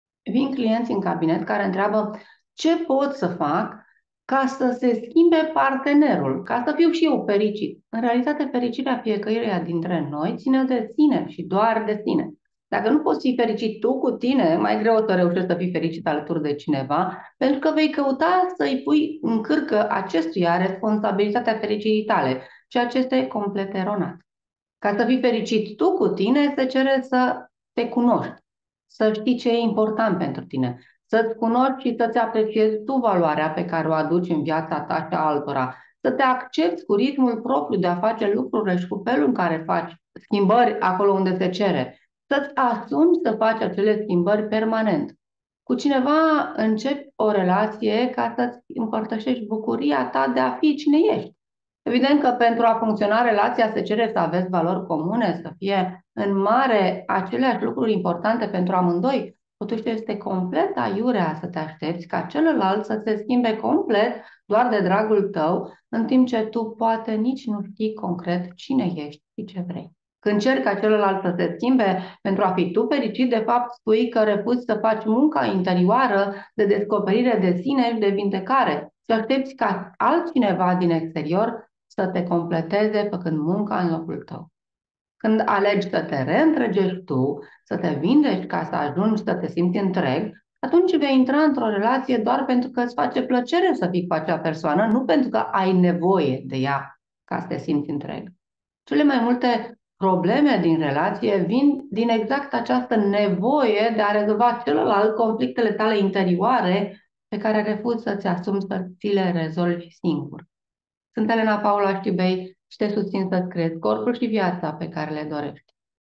Ai nevoie ca celălalt să se schimbe ca să fi fericit?
Vin clienți în cabinet care întreabă ”Ce pot să fac ca să se schimbe partenerul ca să fiu și eu fericit?”. În realitate fericirea fiecăruia dintre noi ține de sine și doar de sine. Dacă nu poți fi fericit tu cu tine mai greu o să reușești să fi fericit alături e cineva pentru că vei căuta să îi pui în cârcă acestuia responsabilitatea fericirii tale, ceea ce este complet eronat. Ca să fi fericit tu cu tine se cere să te cunoști, să știi ce este important pentru tine , să îți cunoști și să îți apreciezi tu valoarea pe care o aduci în viața ta și a altora, să te accepți cu ritmul propriu de a face lucrurile și cu felul în care faci schimbări acolo unde se cere, să îți asumi să faci acele schimbări permanent. Cu cineva începi o relație ca să îți împărtășești bucuria ta de a fi cine ești. Evident că pentru a funcționa relația se cere să aveți valori comune, să fie în mare aceleași lucruri importante pentru amândoi, totuși este complet aiurea să te aștepți ca celălalt să se schimbe complet doar de dragul tău în timp ce tu poate nici nu știi concret cine ești și ce vrei. Când ceri ca celălalt să se schimbe pentru a fi tu fericit de fapt spui că refuzi să faci munca interioară de descoperire de sine și de vindecare și aștepți ca altcineva din exterior să te completeze, făcând munca în locul tău. Când alegi să te reîntregești tu, să te vindeci ca să ajungi să te simți întreg, atunci vei intra într-o relație doar pentru că îți face plăcere să fii cu acea persoana nu pentru că ai nevoie de ea. Cele mai multe probleme din relație vin din exact această nevoie de a rezolva celălalt conflictele tale interioare pe care refuzi să îți asumi să ți le rezolvi singur. Sunt Elena Paula Știubei și te susțin să îți creezi corpul și viața pe care le dorești!